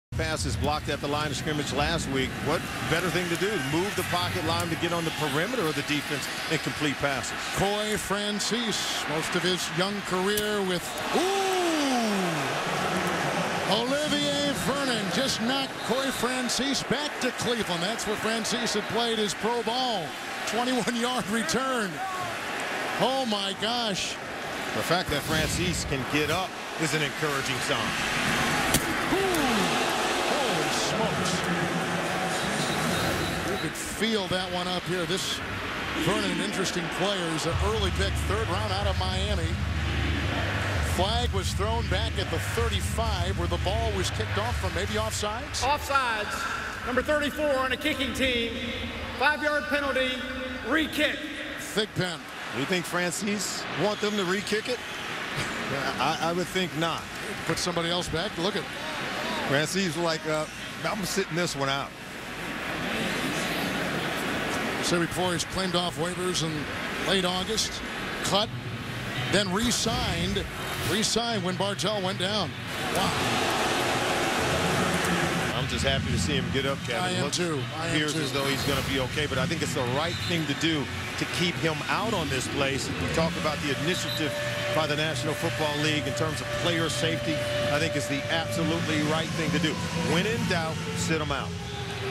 Olivier Vernon knocks Coye Francis back to Cleveland
Viscous by Miami Dolphin Olivier Vernon on Oakland Raiders kick off returner Coye Francis. HD Version